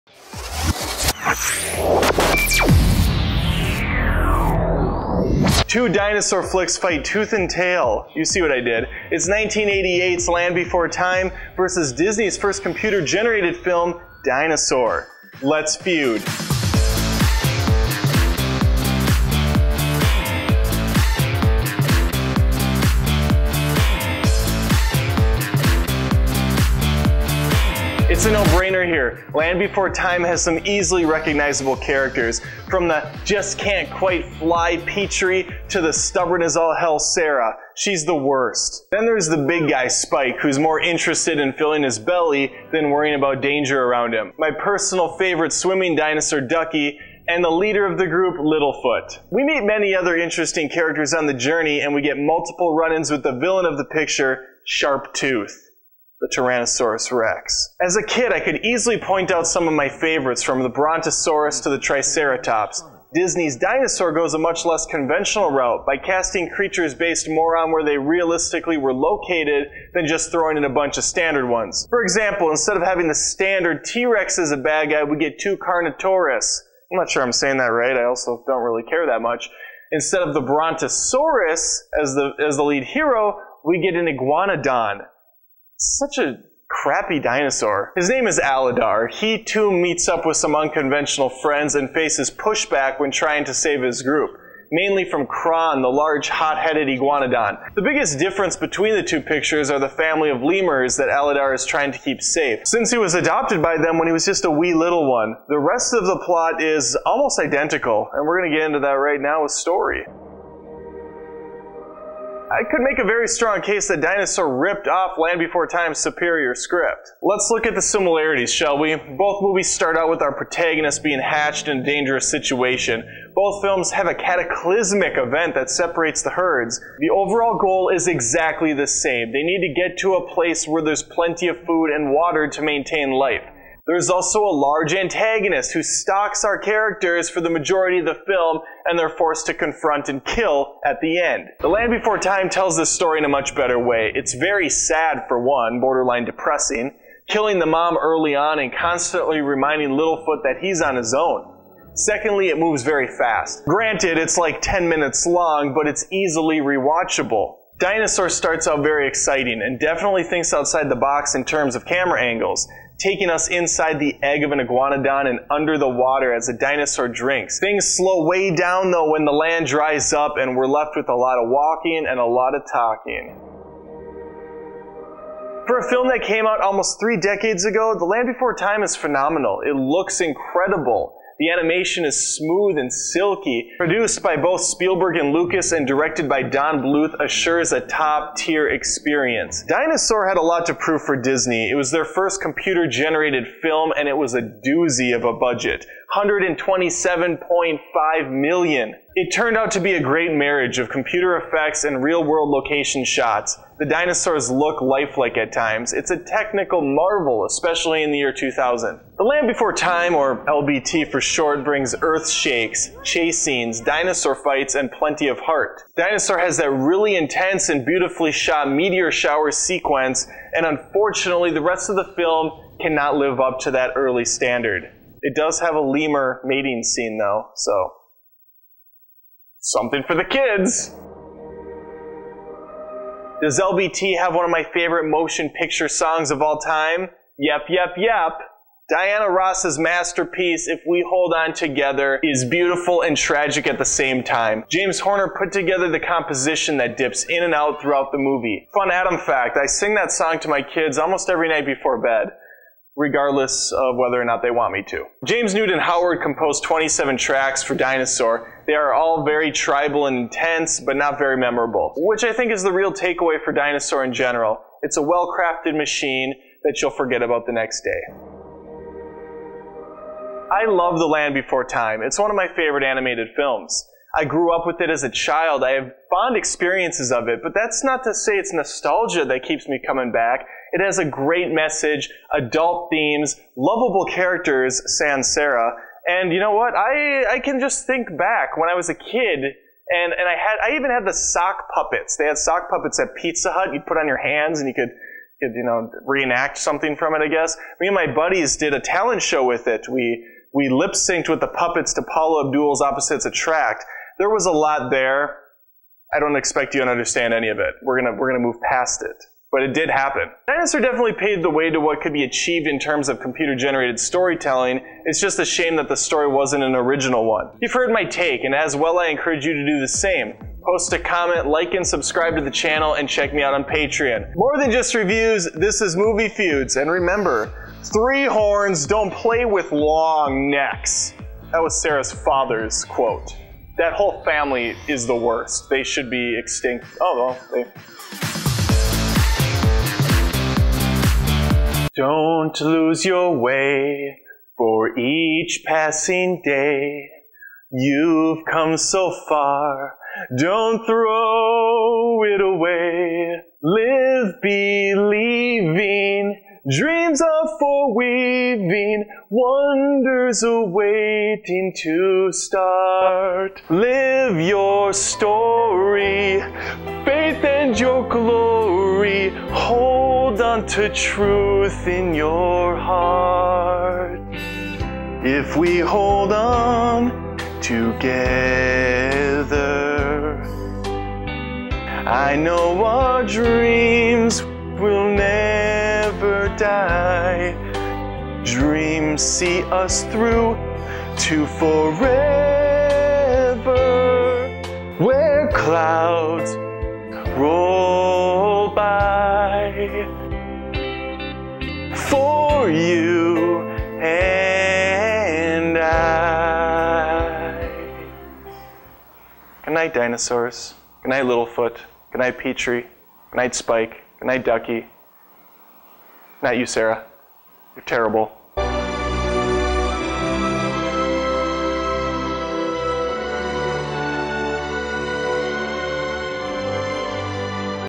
The Land Before Time VS Disney's Dinosaur : Movie Feuds ep133
Two generations of dinosaur flicks fight tooth and tail. It’s 1988’s The Land Before Time VS Disney’s first computer generated film, Dinosaur!

I think it’s a no brainer here. The Land Before Time has instantly recognizable characters from the spastic flyer Petry to the obnoxious and stubborn Sarah. She’s the worst. Then there’s the big guy Spike who’s more interested in filling his belly then worrying about danger around him. Then there’s our swimmer Ducky, my personal favorite and the leader of the group Little Foot. We meet other interesting characters on their journey together and get multiple run-ins with the villain of the picture, Sharp Tooth.

As a kid I could easily point out some of my favorites from the Brontosaurus to the Triceratops. Disney’s Dinosaur goes a much less conventionally route by casting creatures based more on where they realistically where located then just throwing in a bunch of more standard ones. Instead of the T-Rex we get two Carnotaurus, instead of the Brontosaurus lead we have an Iguanodon named Aladar. He too meets up with some unconventional friends and faces pushback when trying to save his group. Mainly from Kron, the large, hot-headed Iguanodon. The biggest difference between the two pictures are the family of Lemurs that Aladar is trying to keep safe since he was adopted by them when he was an infant. The rest of the plot is essentially identical and we will get to that right now.
Check out the video for the full breakdown.